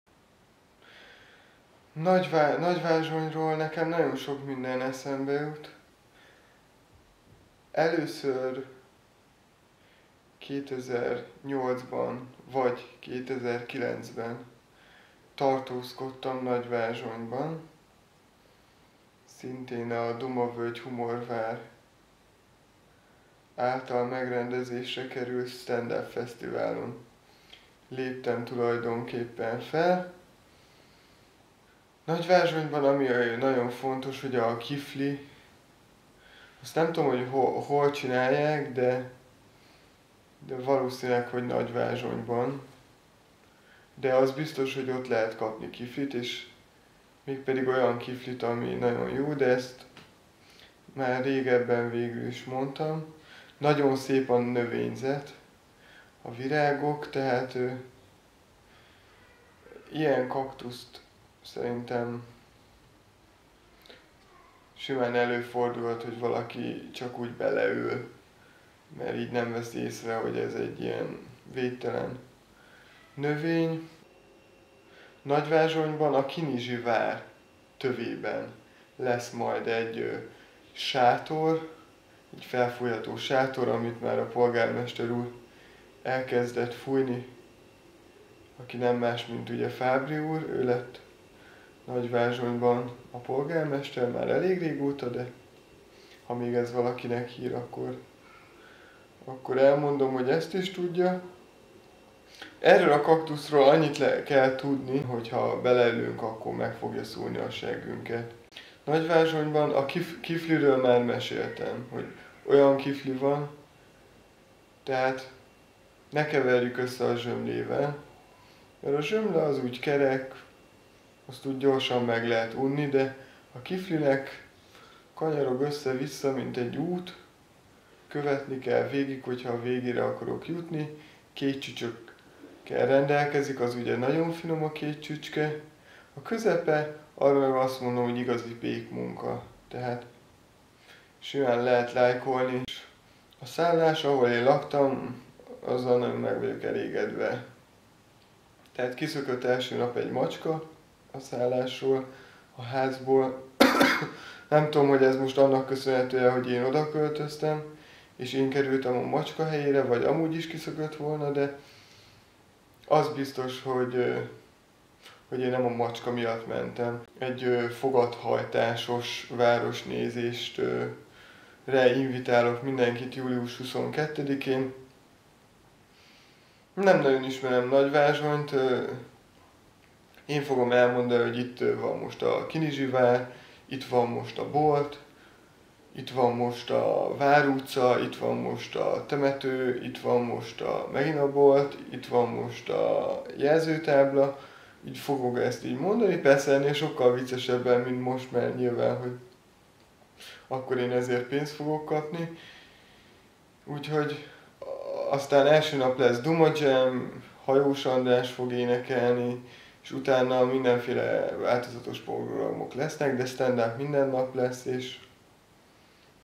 Hajdú Balázs nagyvázsonyi élményei
Hajdú Balázs kedvet csinál a 2011-es OTP Dumavölgy-Humorvárhoz Ne maradj le a poénról!